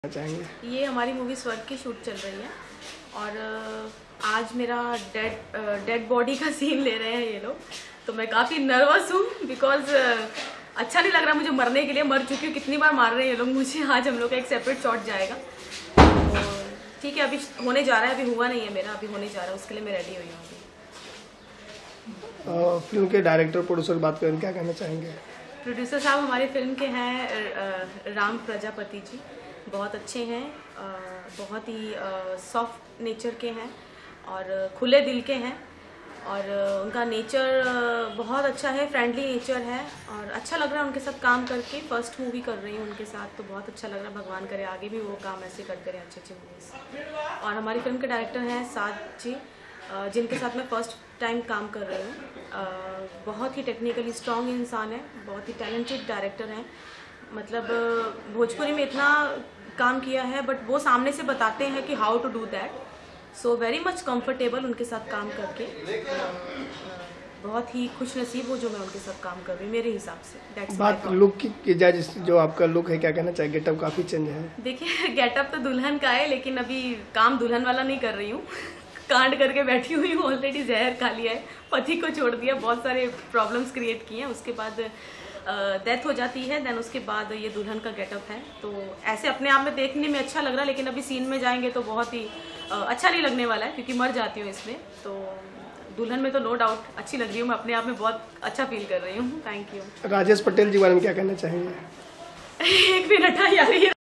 Interview Actress Priya Sharma On Lacation Shooting Bhojpuri Movie SWARG Iस्वर्ग I
Singer, Actor,  Bhojpuri Movie Swarg On LOcation Shooting At Mumbai.
Banner: Ram intertainment, Producer: Ramawadh V Prajapati, Director: Shad Kumar, Writer: Lalji Yadav, Music: Chhote Baba. Starcast: Arvind Akela Kallu, Aditya Mohan, Priya Sharma, Nisha Dubey, Abhilasha, Sanjay Pandey, Kiran Yadav, Gopal Rai, Anoop Arora, Palavi etc.
Camraman: Nandlal Chaudhri, Art Director: Nazir Khan, Dance Master: Kanu Mukherjee
Fight: Heera yadav.